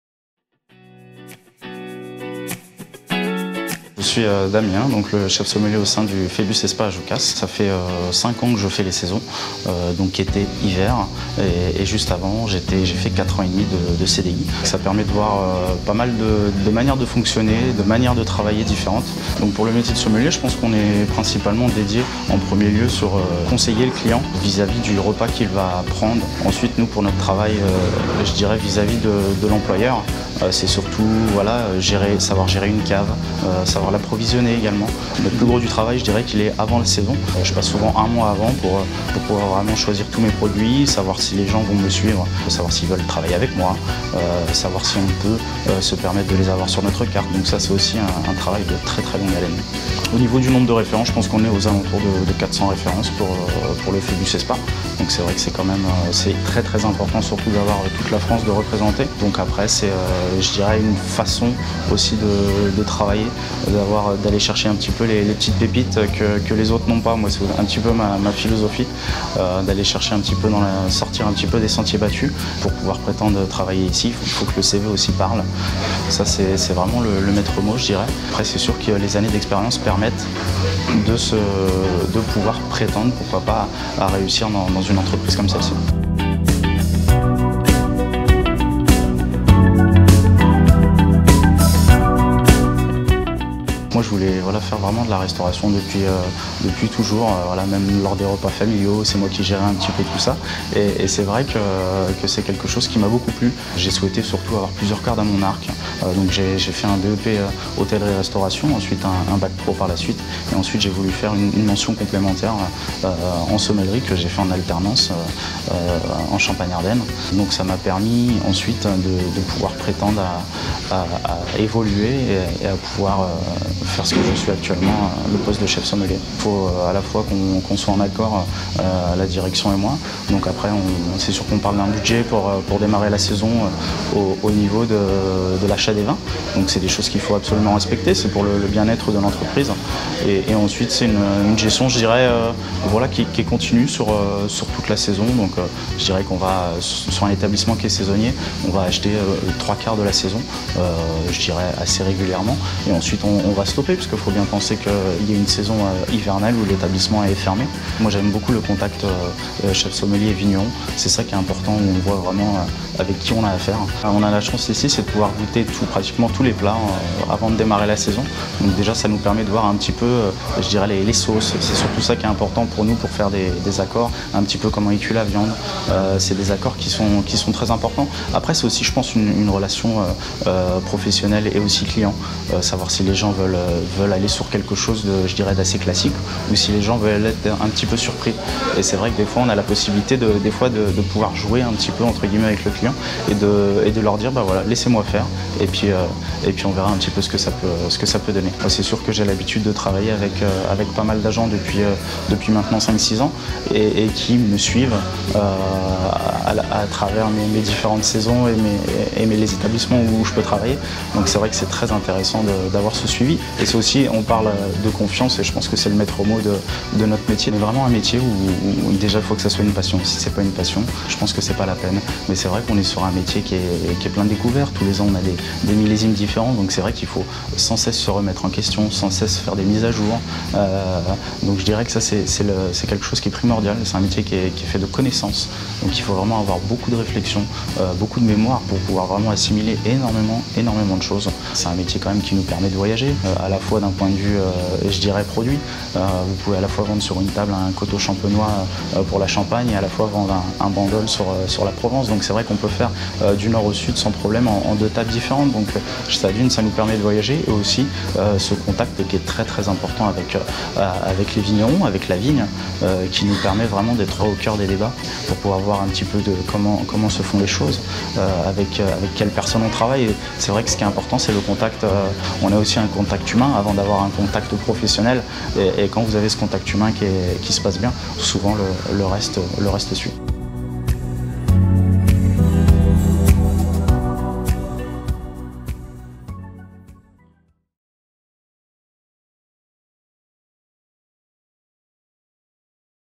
Le métier de sommelier
Découvrez le métier de sommelierdans le secteur des hôtels, cafés, restaurants.

Sur la plateforme ICICESTMAPLACE, de nombreuses opportunités de formation et d’emploi sont présentés pour aider à préparer son avenir ou envisager une reconversion professionnelle.